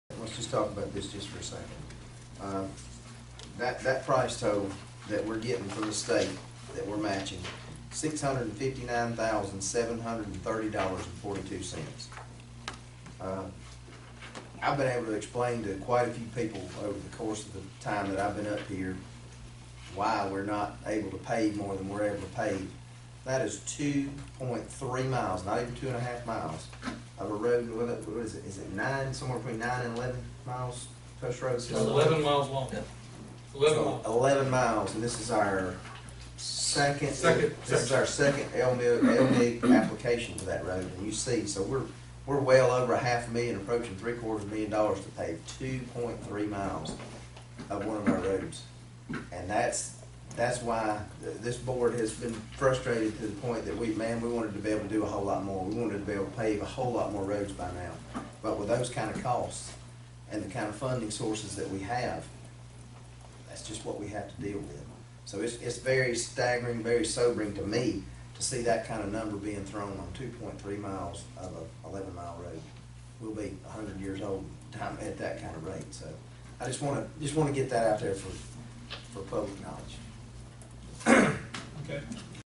Commissioner Luke On Why The County Doesn't Pave More Roads
Doug Luke stresses the impact road construction costs have on the ability of the County to properly maintain our roads.